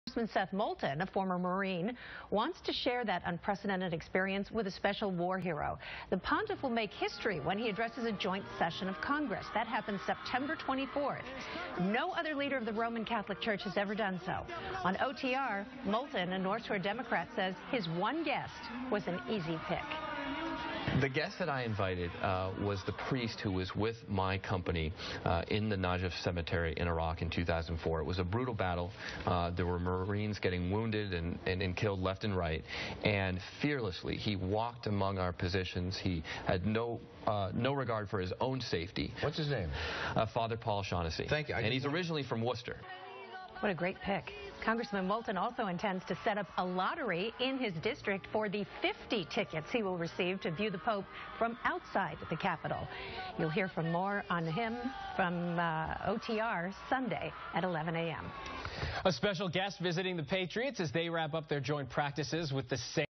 WCVB: Moulton Invites Father Paul J. Shaughnessy to Pope's Address
ABC Channel 5 in Boston reports on Congressman Moulton's invitation to Father Paul J. Shaughnessy to attend Pope Francis' address to a joint session of Congress in September